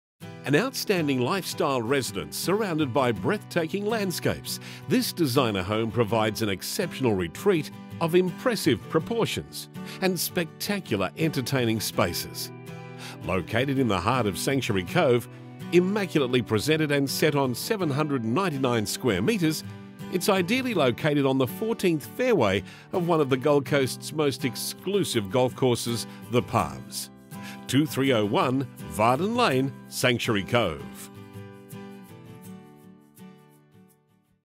2301 Vardon Lane, Sanctuary Cove QLD 4212
An outstanding lifestyle residence surrounded by breathtaking landscapes, this designer home provides an exceptional retreat of impressive proportions and spectacular entertaining spaces. Located in the heart of Sanctuary Cove, immaculately presented and set on 792sqm, it's ideally located on the fourteenth fairway of one of the Gold Coasts most exclusive golf course, The Palms.